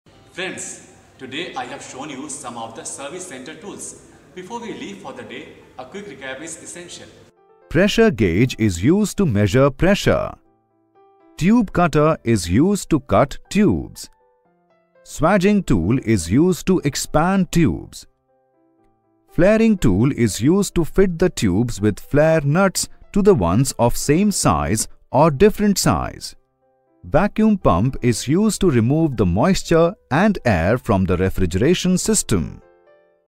Summary for the Types of Service Centre Tools
This video shares brief summary about the different types of Service Centre Tools.